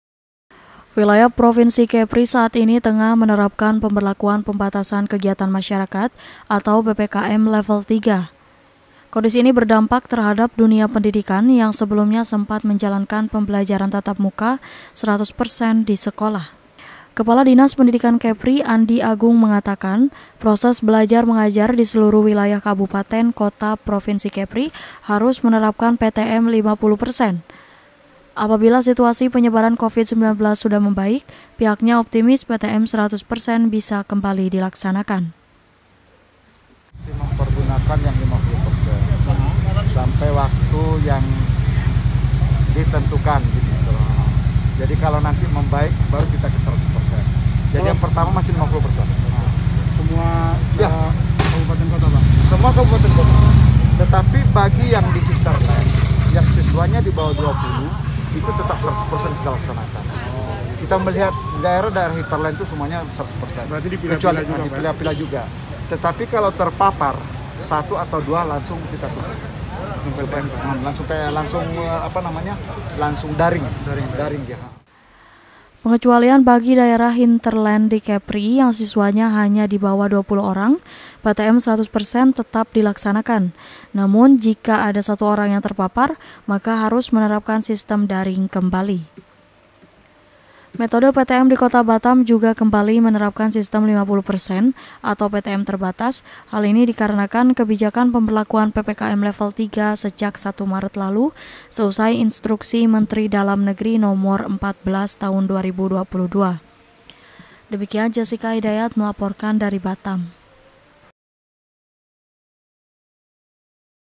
PTM DI KEPRI KEMBALI DILAKSANAKAN SECARA TERBATAS
SEIRING PENINGKATAN  KASUS COVID 19 DI SEJUMLAH KABUPATEN-KOTA /   PROVINSI KEPRI KEMBALI MENERAPKAN PPKM LEVEL 3  / HINGGA 14 MARET 2022 MENDATANG // TERKAIT HAL TERSEBUT / KEGIATAN PEMBELAJARAN TATAP MUKA (PTM) PUN JUGA HARUS KEMBALI DILAKSANAKAN SECARA TERBATAS / DENGAN 50 PERSEN DARI TOAL SISWA PER KELAS//